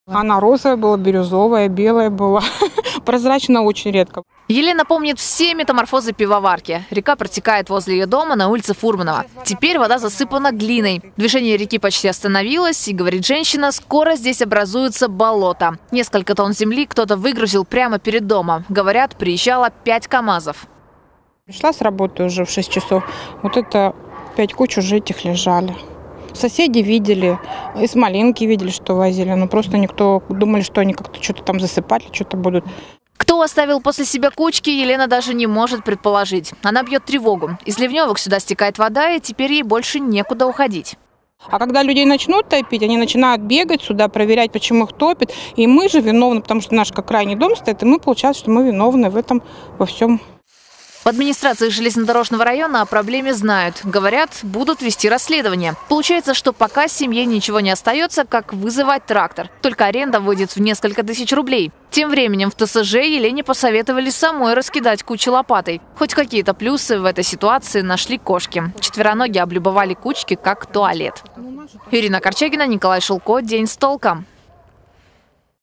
Неизвестные засыпали берег Пивоварки огромными кучами глины
Загадочные кучки появились на улице Фурманова. К нам в редакцию обратилась женщина, которой под окна вывалили несколько тонн земли. Жительница частного дома не знает, что теперь делать с таким «подарочком», ведь кучи мешают сливу воды, к тому же, рядом протекает река Пивоварка, которая тоже оказалась засыпанной.